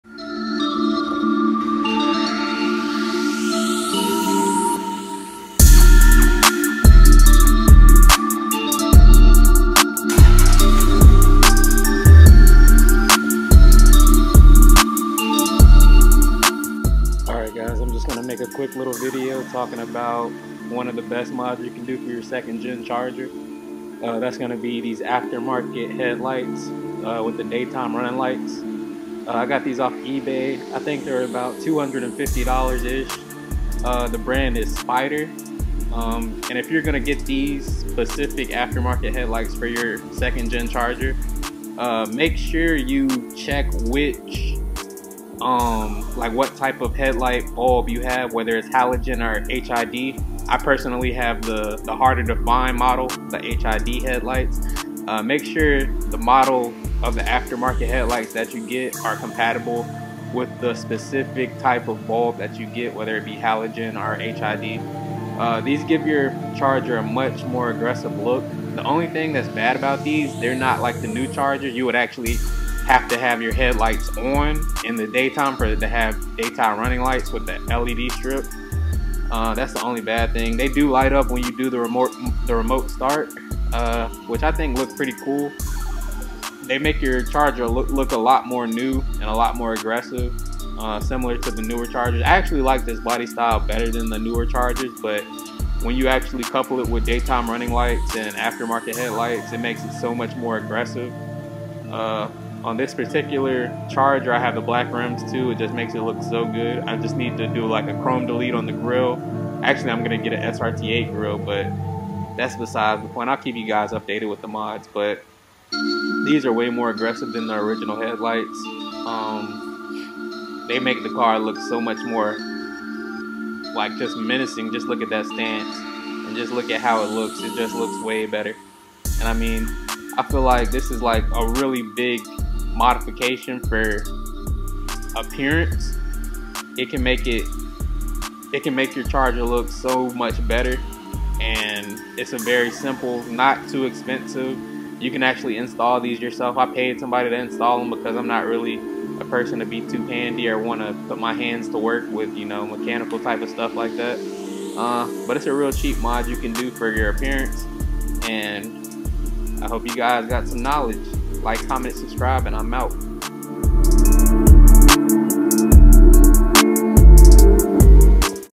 charger aftermarket headlights
just talking about the aftermarket headlights on my gen 2 charger.